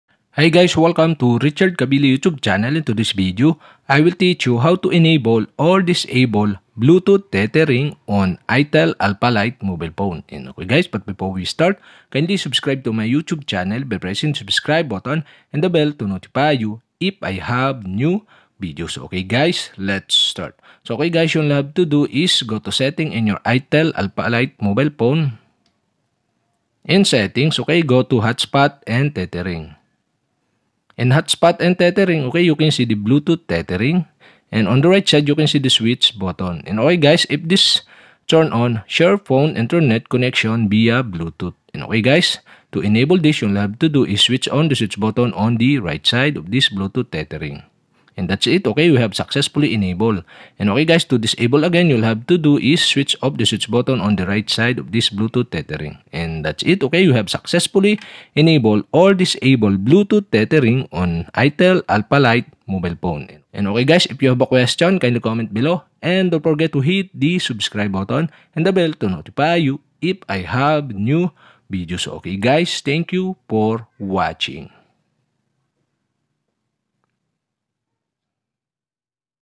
How to enable/disable bluetooth tethering on itel Alpha Lite
Do you want to enable or disable bluetooth tethering on itel Alpha Lite mobile phone.

Thanks...